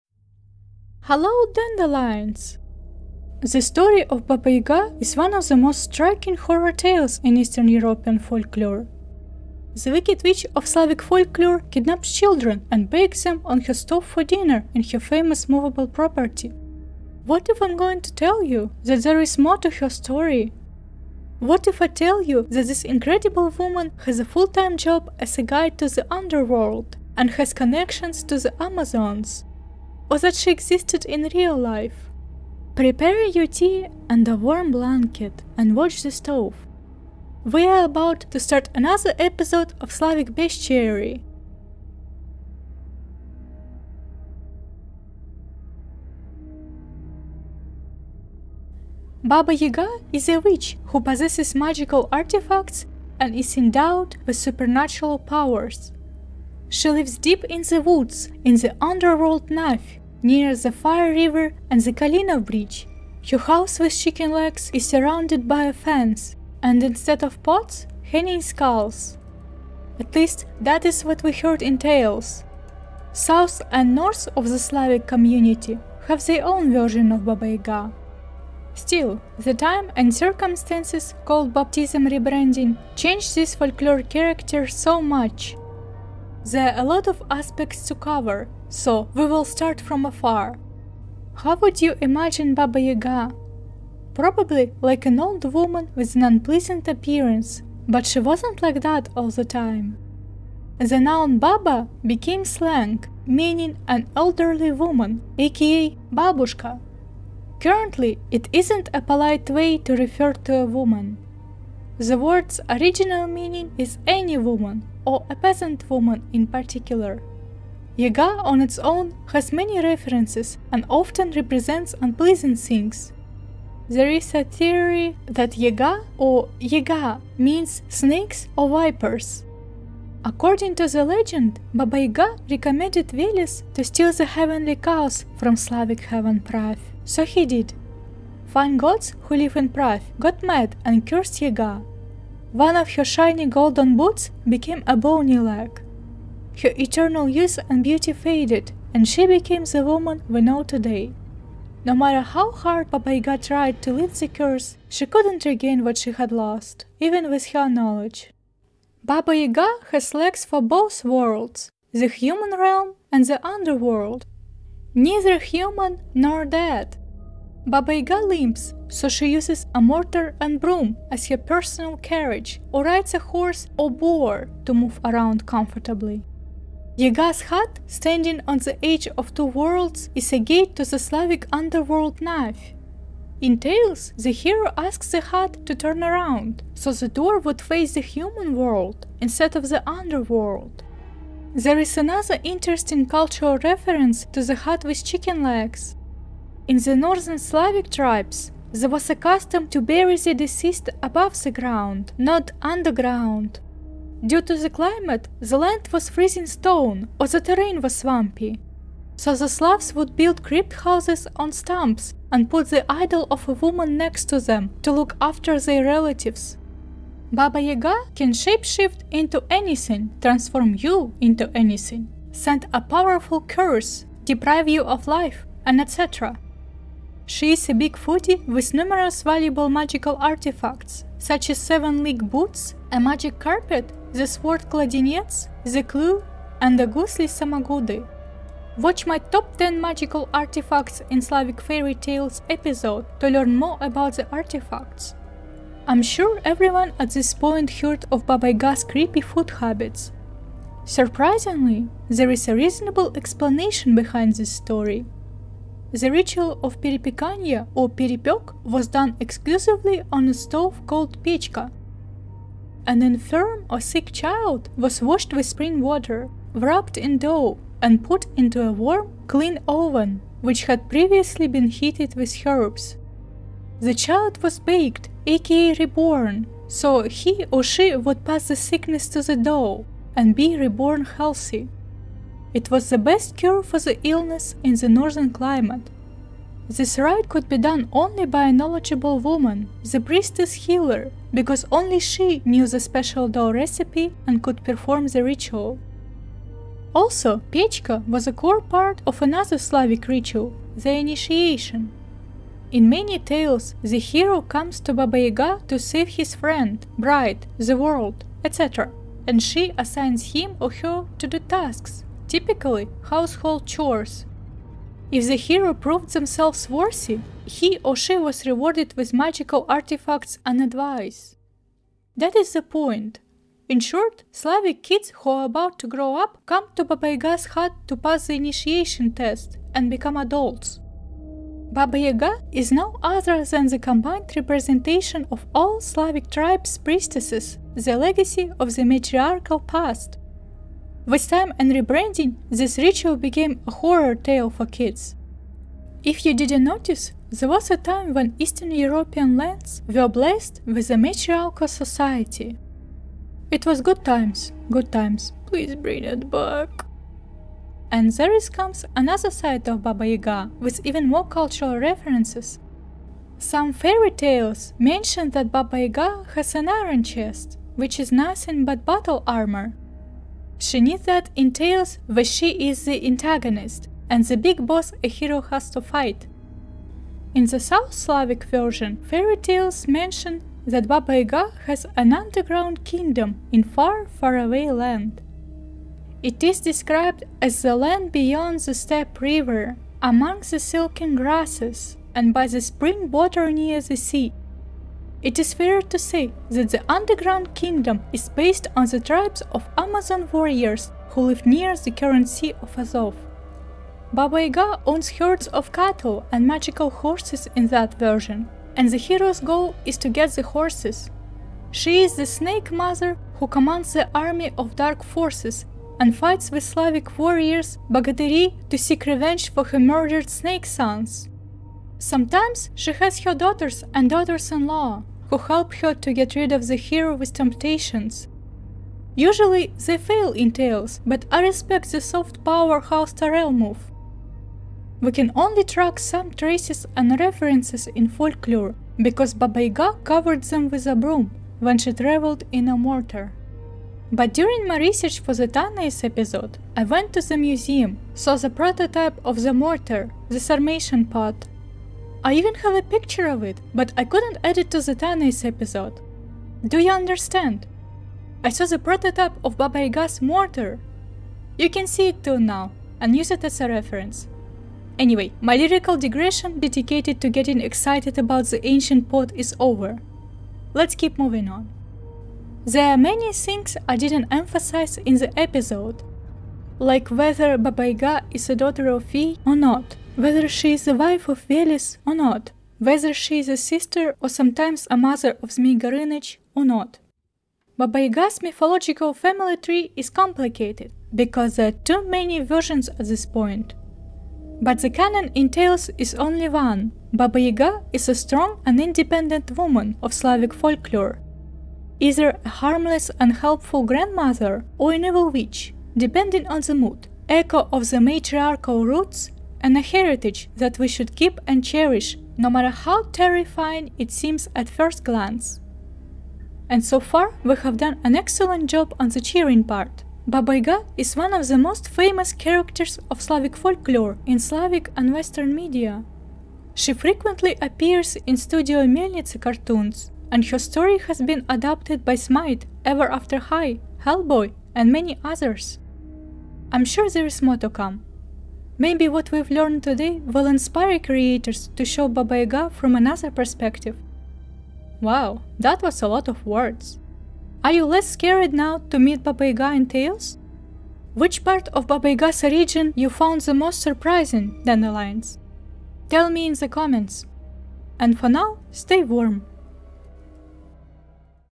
The VERY Messed Up Origins™ of Baba Yaga, the Bone Leg | Slavic mythology
Every Slavic child is familiar with the name Baba Yaga and her house with chicken legs at the edge of a forest. The Slavic witch has also known as Baba Yaga the Bone Leg because she has only one leg, though many tales tell why. This video explores her origins and summarizes how she became the person she is today.